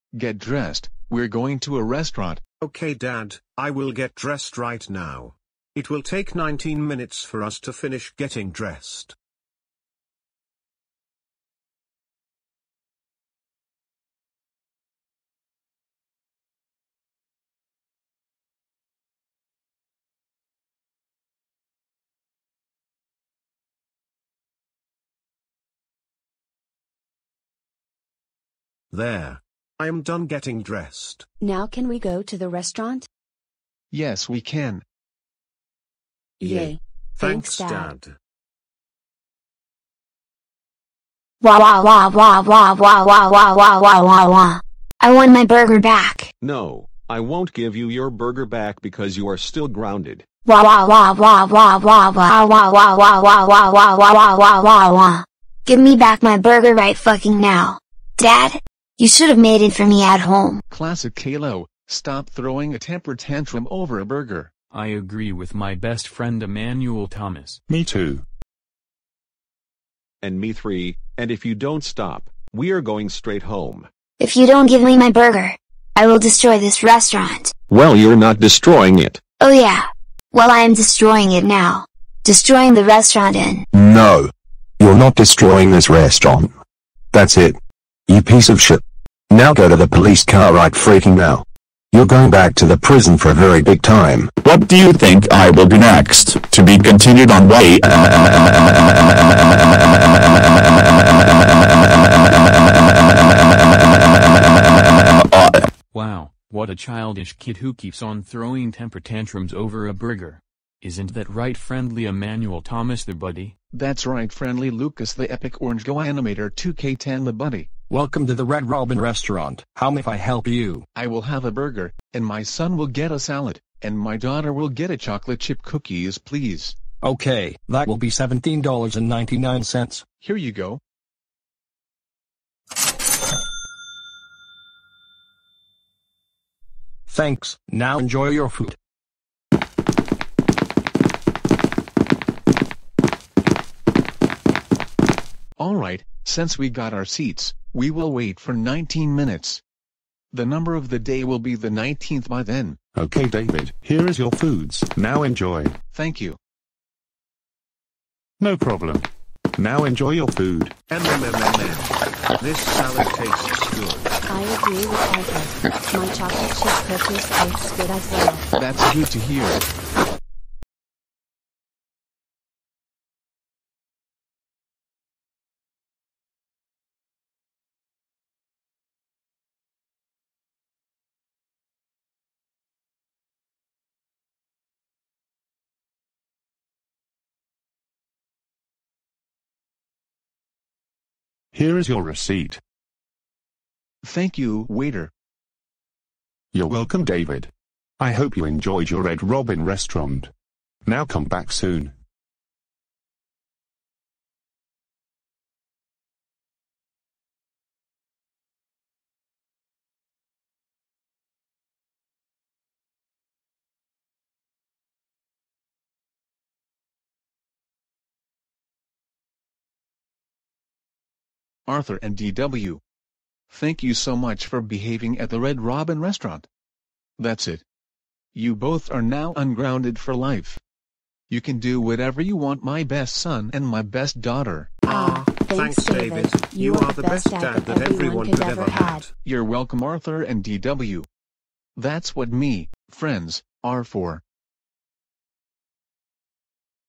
Arthur And D.W. Behaves At The Restaurant And Gets Ungrounded
Inspired By: David Young